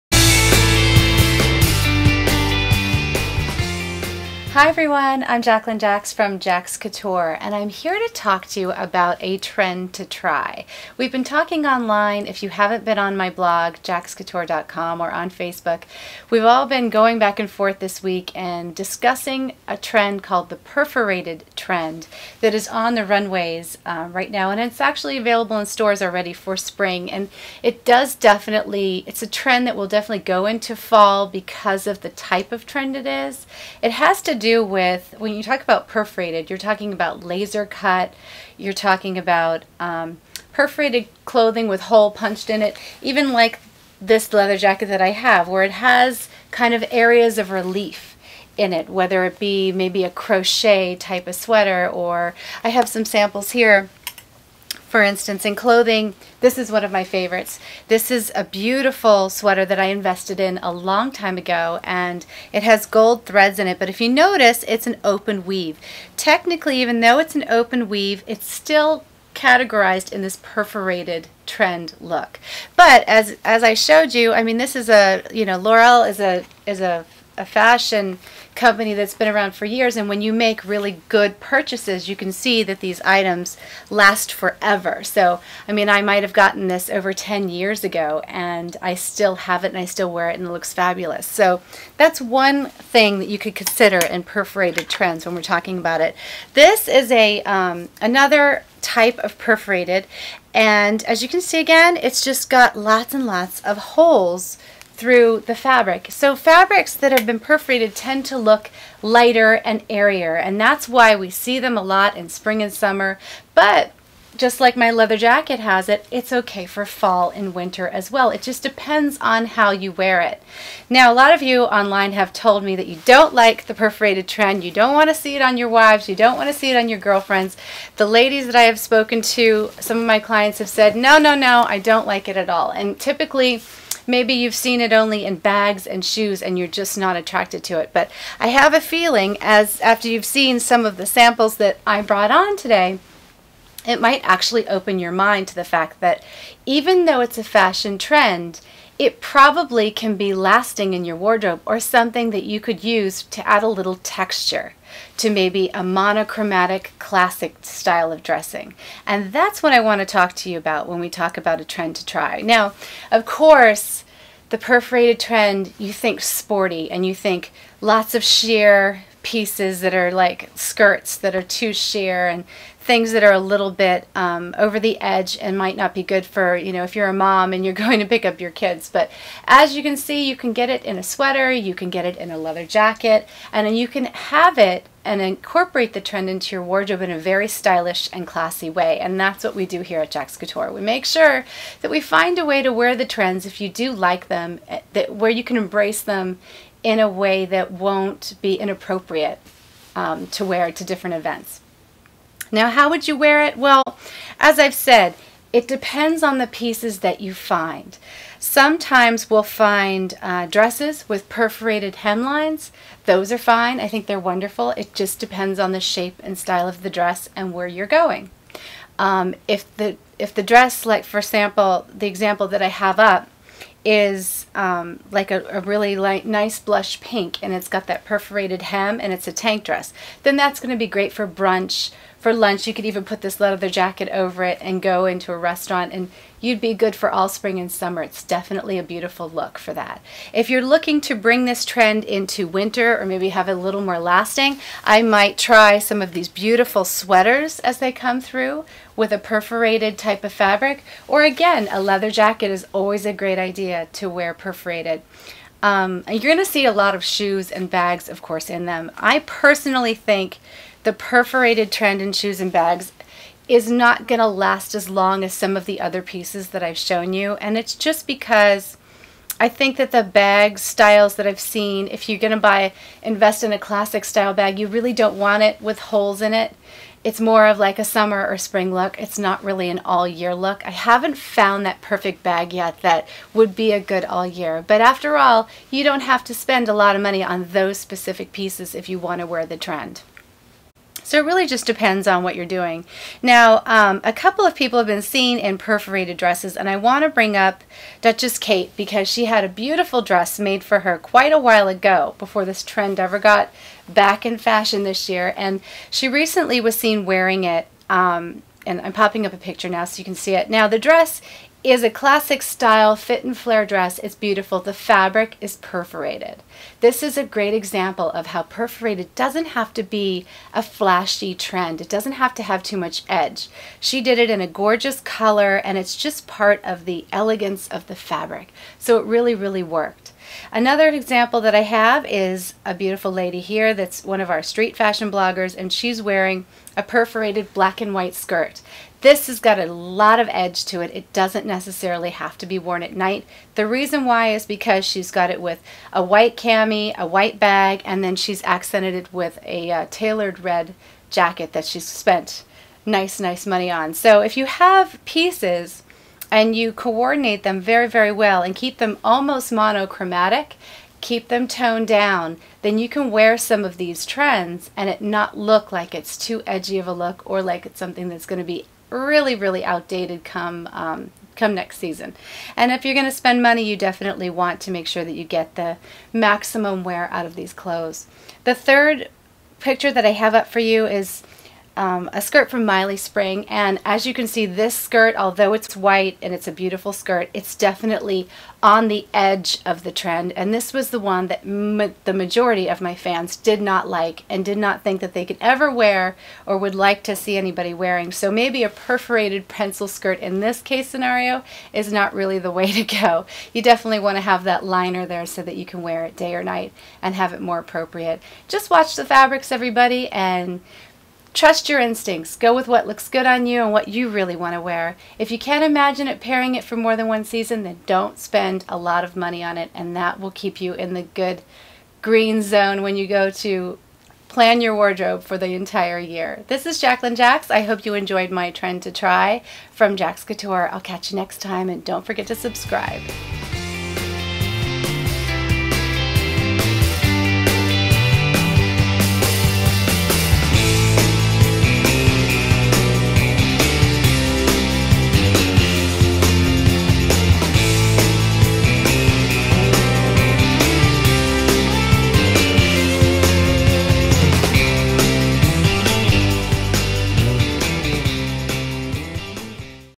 Trend To Try Perforated Fashion Skirts, Dresses, Shoes, and Bags for Spring
Whether pierced, hole punched, or laser-cut, perforated clothing and accessories are having a major moment this season. This unexpected look is a trend you should definitely try. Perforated pieces are playing a prominent role in both Spring and Fall collections of many designers so there is no doubt we will see this style have some staying power. With a sporty-meets-punk vibe, these cut clothing and accessories make a strong style statement, adding a visually interesting touch to any look. The treatment to the fabric makes everything lighter in appearance and physical weight making it a great option for spring and summer as well. In my opinion, it's a trend that offers a nice texture to classic shapes that are handy to have in your wardrobe. Take a look at some of the styles and tell me how you feel about it. -JAX

- Including Princess Kate Middleton wearing the trend
"The Duchess of Cambridge Style"